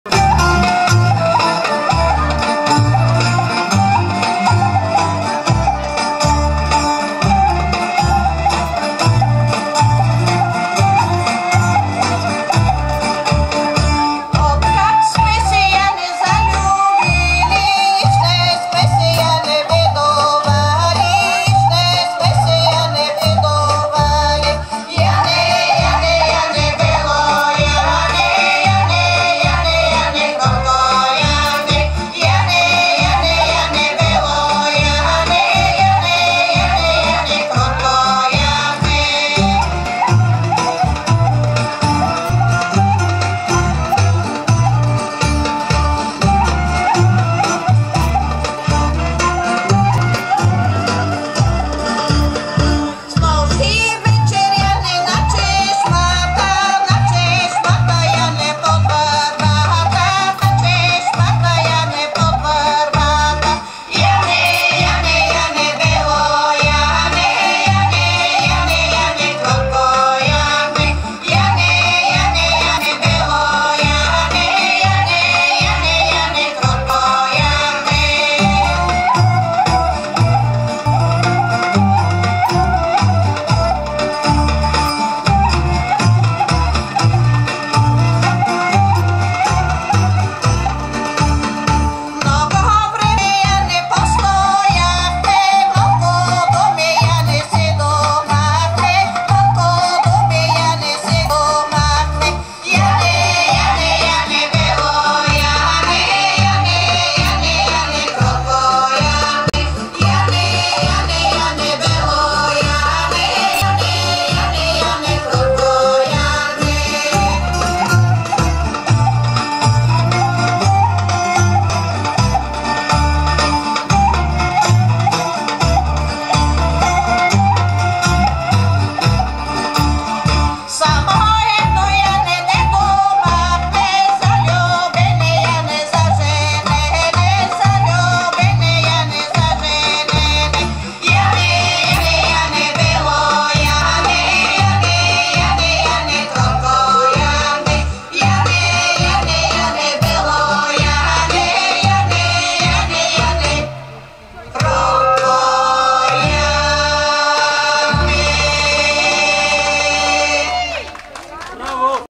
Самодейци от с. Горна Гращица, общ. Кюстендил - 18.07.2021 г.
IV фолклорен събор в чест на Васил Левски - "Новоселяне пее"